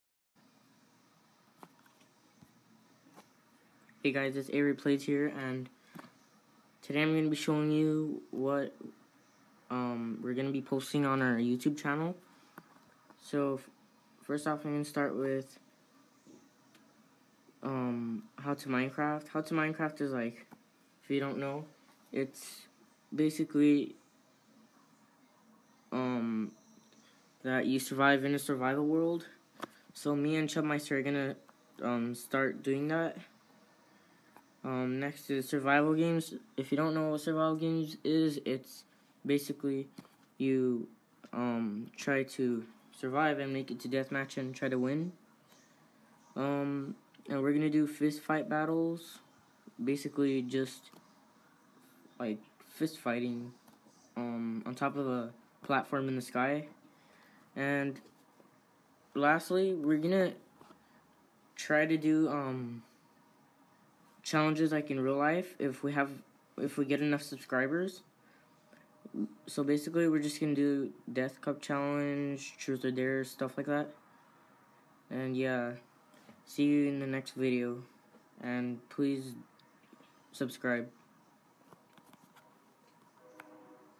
Introduction Of YouTube Channel
What's included in your channel you ask? Fist fight battles, Survival games, How to minecraft, and challenges in real life!!!